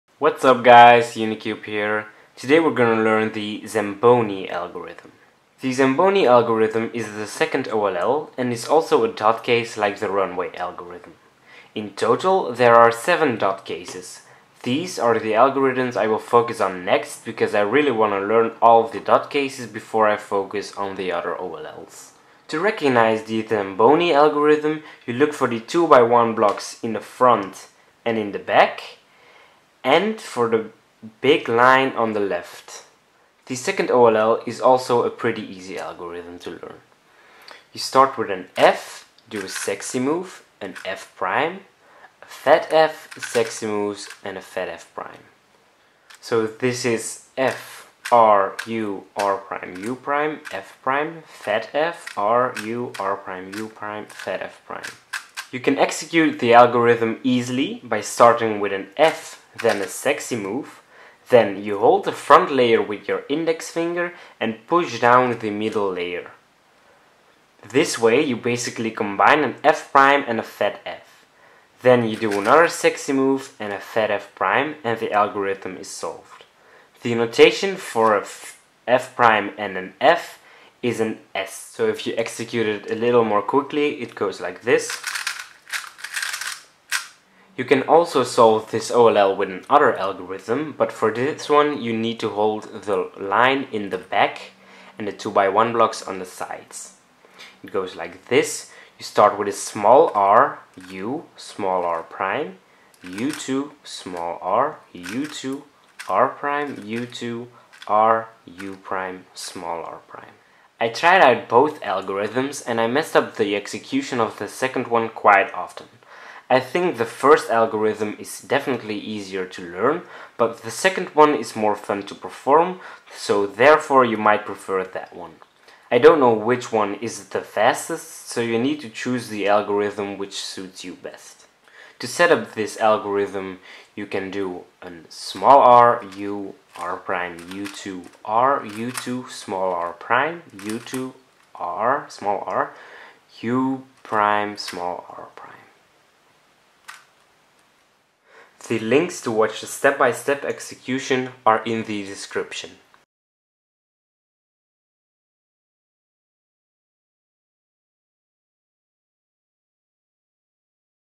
Learn OLL with me! - OLL 2 - Zamboni
Learn OLL with me! I want to learn full OLL, and whenever I learn an algorithm I'm gonna make a video. This way you can learn all 57 OLL algorithms.

Today we'll learn the Zamboni algorithm (OLL 2).

Set-up: r U R’ U2 R U2 r’ U2 r U’ r’
Zamboni 1: F (R U R' U') S (R U R' U') f'
Zamboni 2: r U r' U2 r U2 R' U2 R U' r'